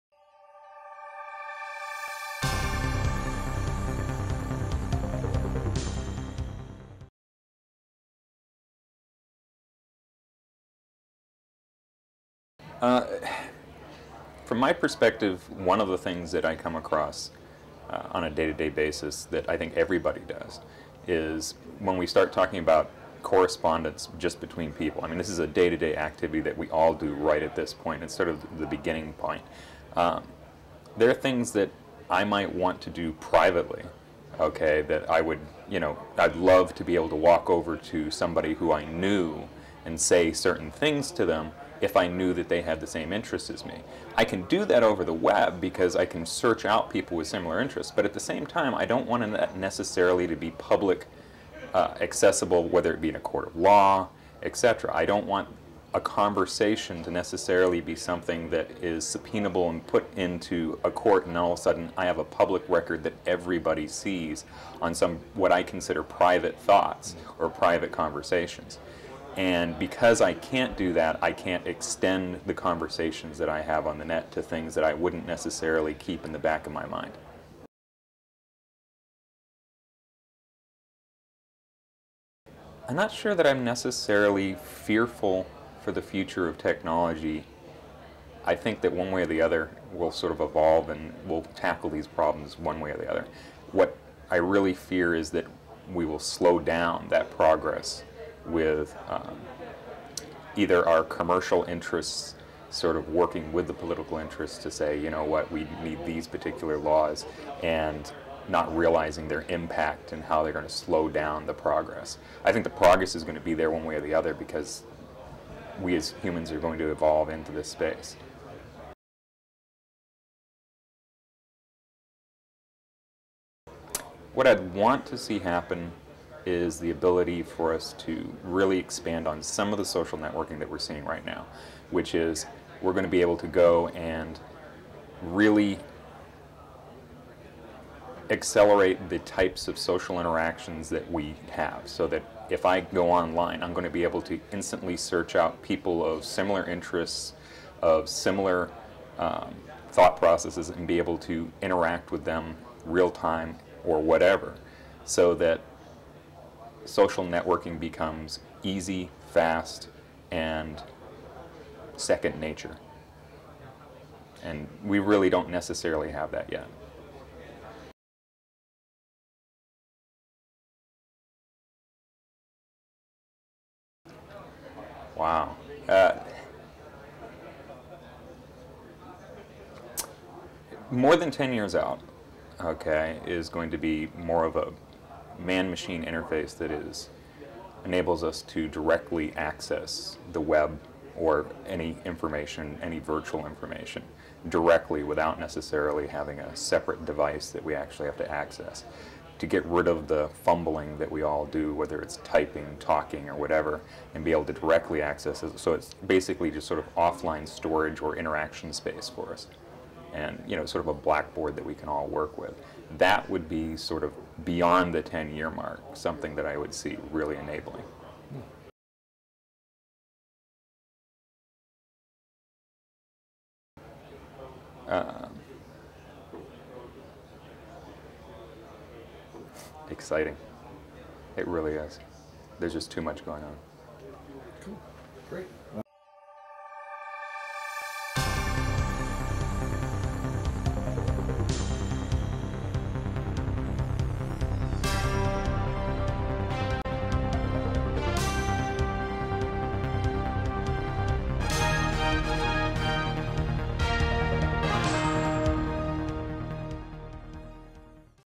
The Metaverse Roadmap Summit Interviews, 2006: Marty Poulin
This was recorded at Stanford Research Institute International in Menlo Park, California, in May 2006 at the Metaverse Roadmap Summit. Recorded by the Imagining the Internet Center, a non-profit, global-good initiative of Elon University, located in North Carolina, USA,